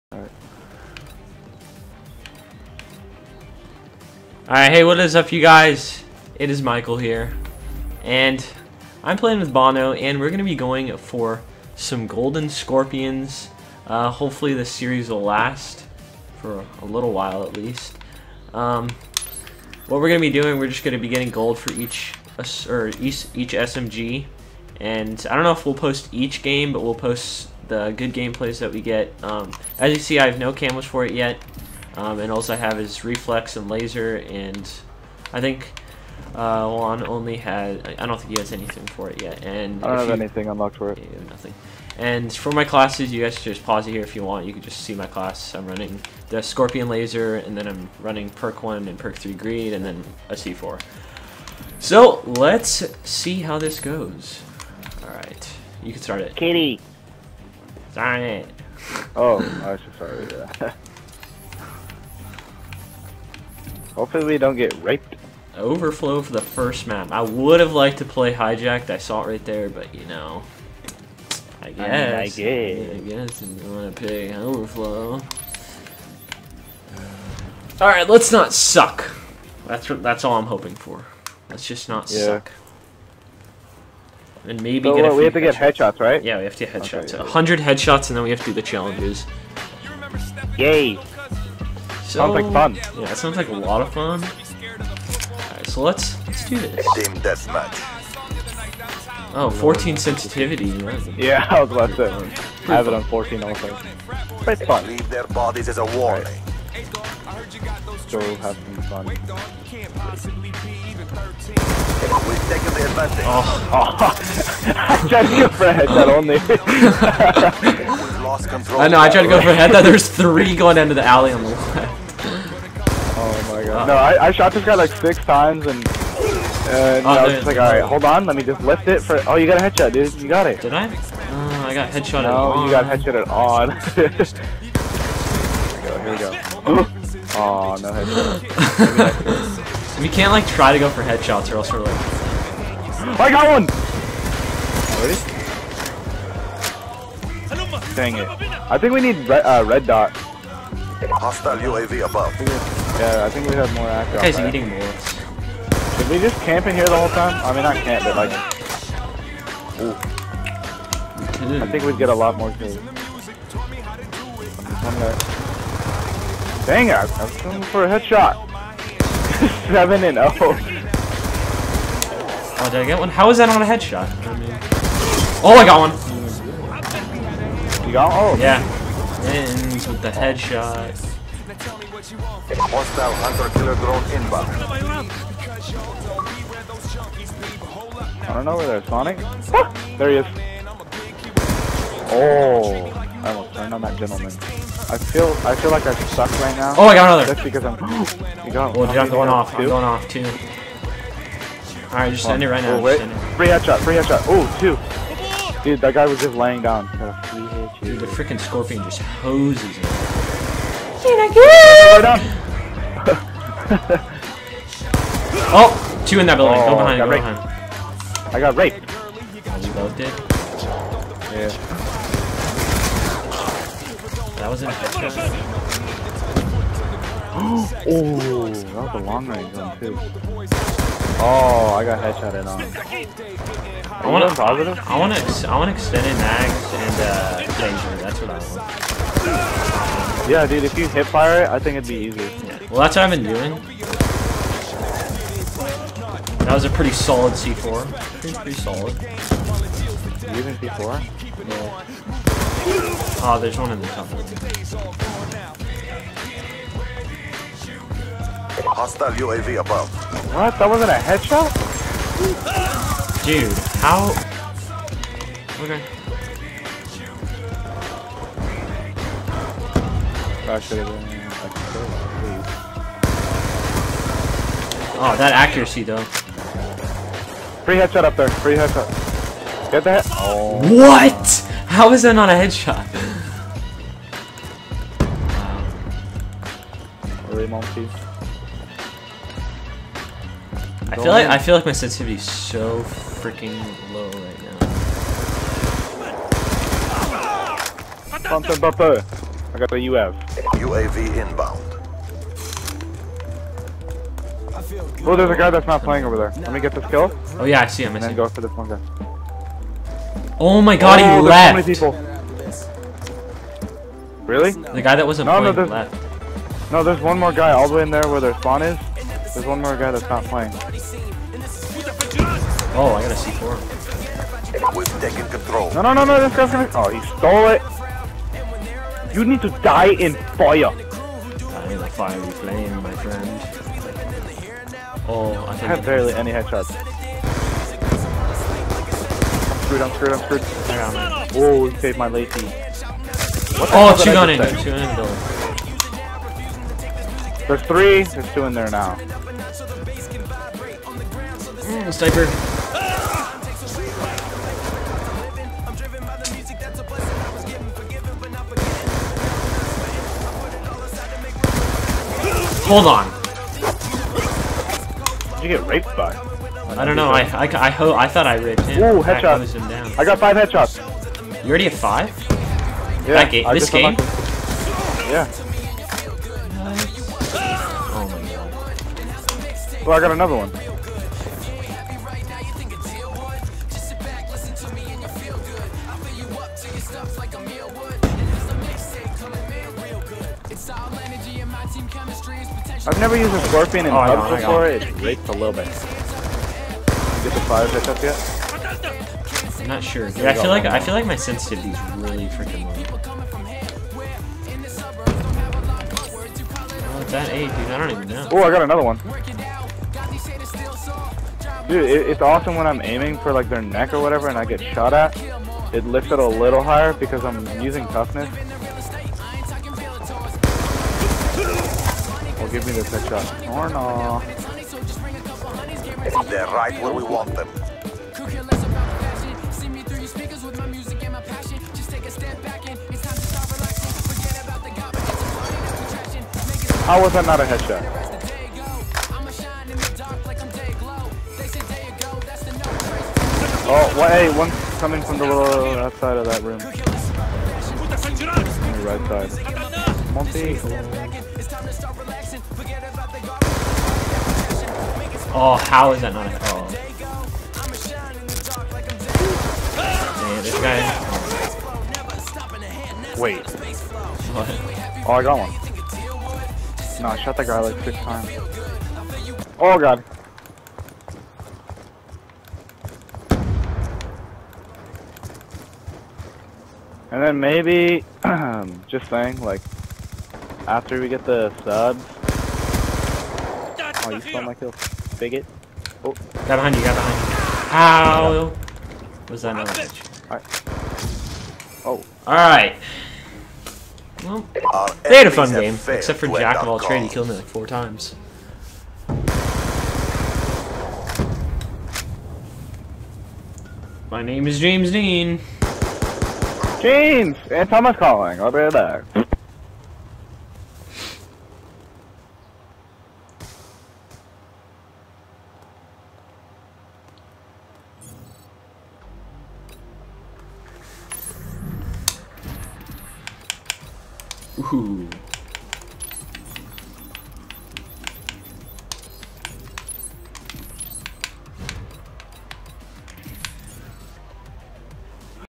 Road to Scorpion Gold - Episode 1
Hey guys hope you enjoy part 1 of my scorpion to gold watch with BANO! Hopefully this series will stick.